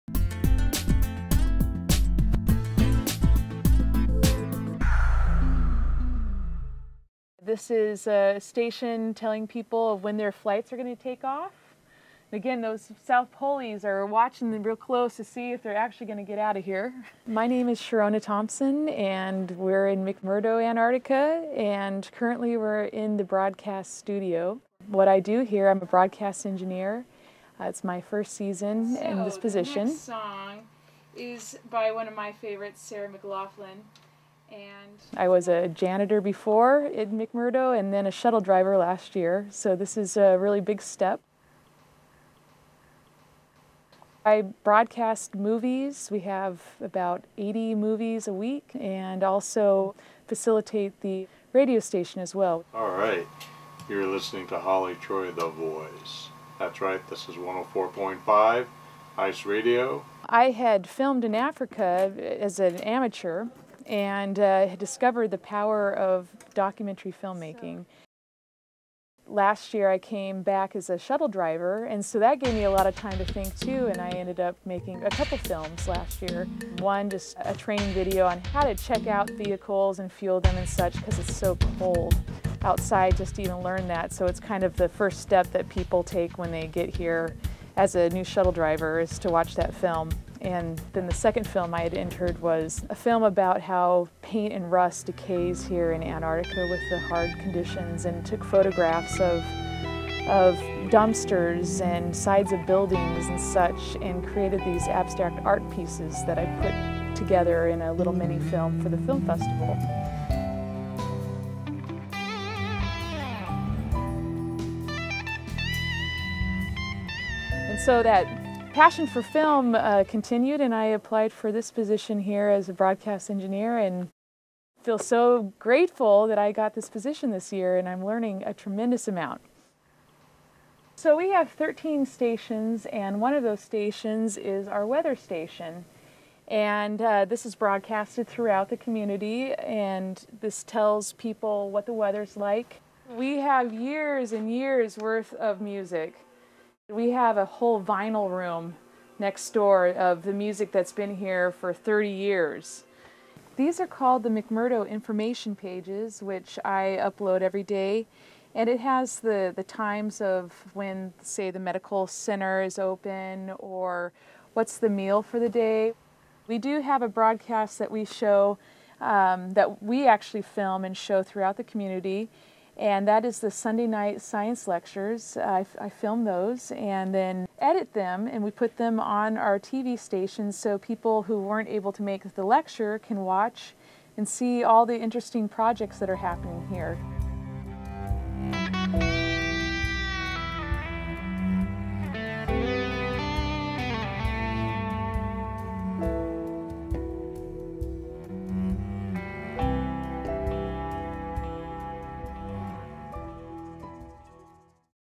Sharona Thompson, Antarctica Broadcast Engineer
Antarctica Broadcast Engineer Sharona Thompson works at McMurdo Station, the largest  of the U.S. Antarctic bases.